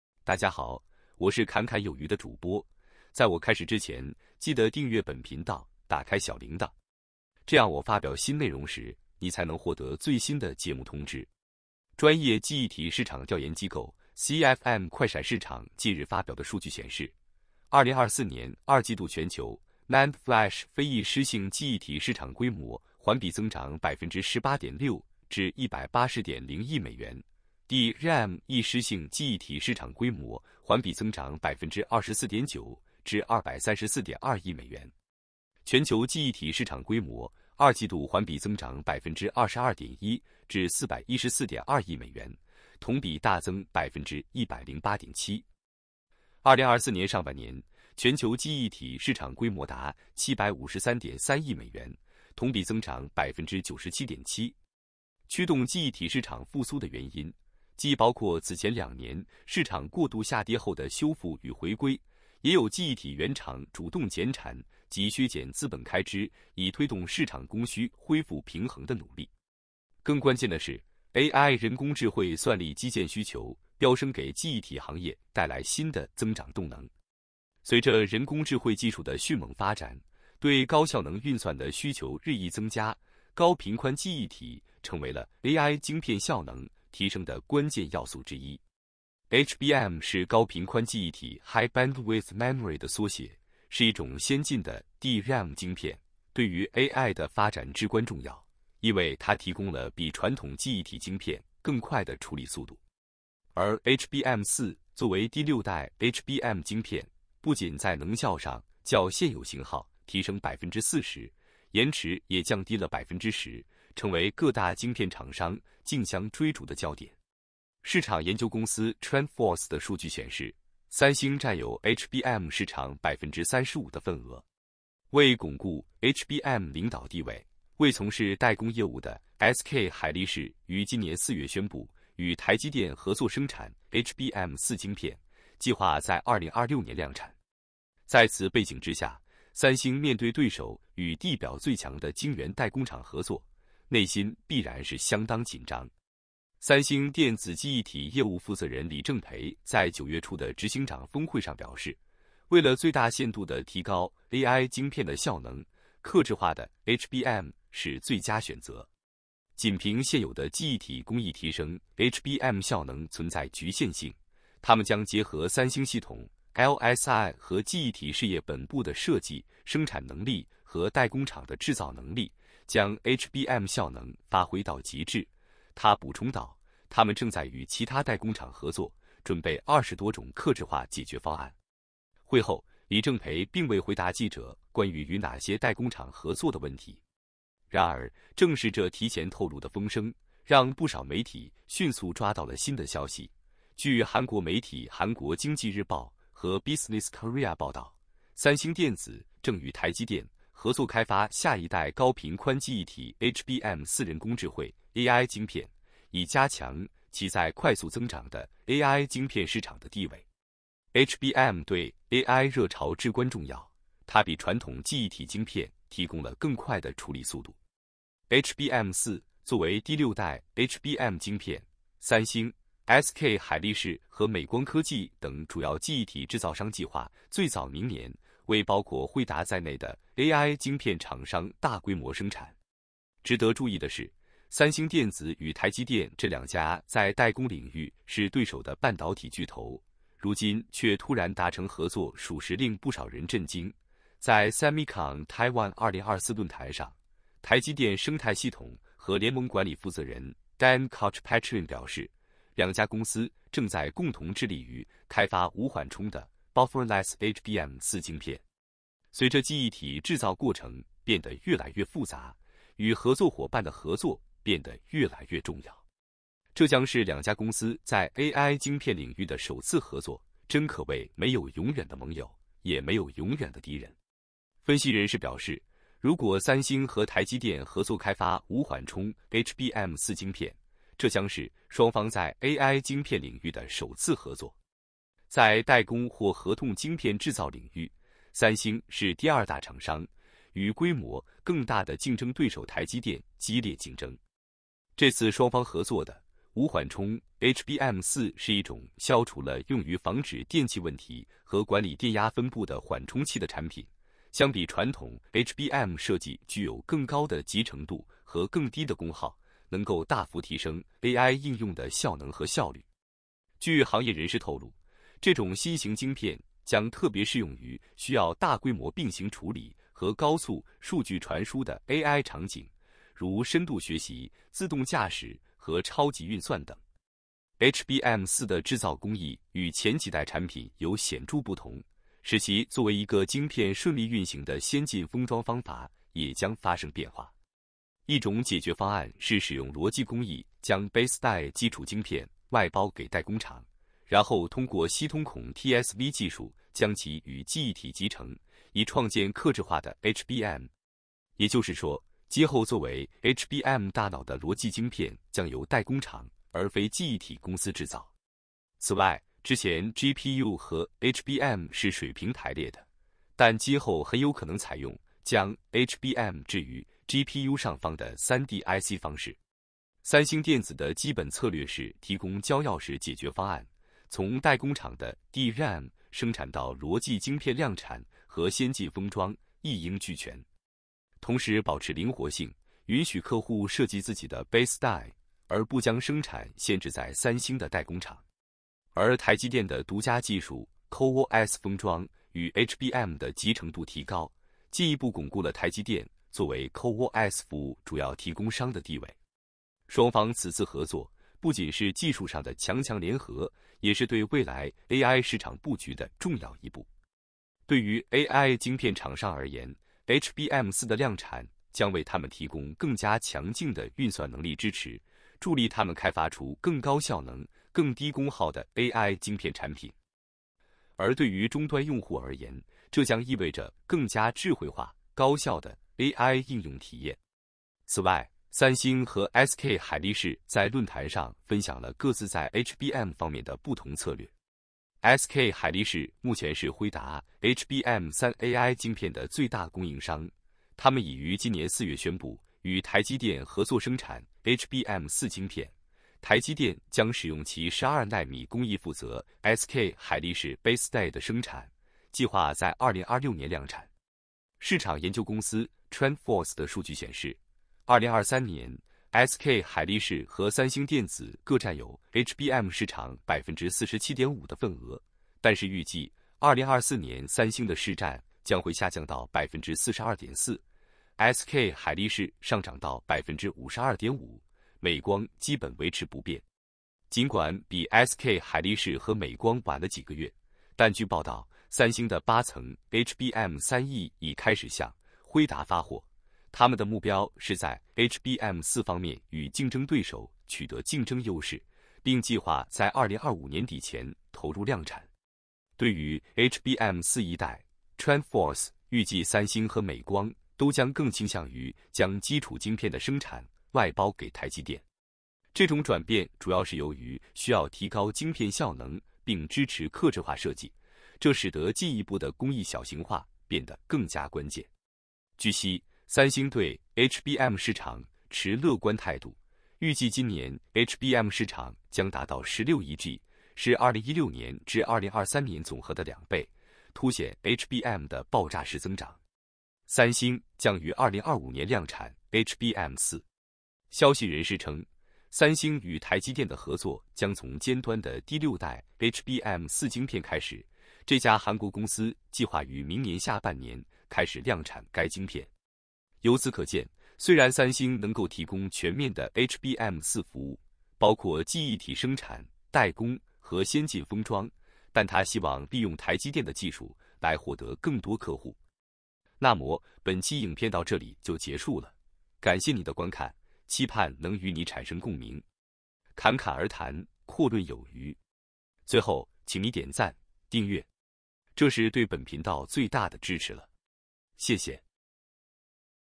極破天荒！三星與台積電首次達成巨合作，攜手開發頂級無緩衝HBM4 AI晶片。已對自家晶圓代工廠喪失信心，製造過程越來越複雜，對AI熱潮至關重要。緊咬SK海力士和美光，HBM3e開始向輝達發貨。
隨著人工智慧技術的迅猛發展，對高效能運算的需求日益增加，高頻寬記憶體成為了AI晶片效能提升的關鍵要素之一。HBM是高頻寬記憶體High Bandwidth Memory的縮寫，是一種先進的DRAM晶片，對於AI的發展至關重要，因為它提供了比傳統記憶體晶片更快的處理速度。據韓國媒體《韓國經濟日報》和《Business Korea》報道，三星電子正與台積電合作開發下一代高頻寬記憶體HBM4人工智慧AI晶片，以加強其在快速增長的AI晶片市場的地位。市場研究公司TrendForce的數據顯示，2023年，SK海力士和三星電子各佔有HBM市場47.5%的份額，但是預計2024年三星的市佔將會下降到42.4%，SK海力士上漲到52.5%，美光基本維持不變。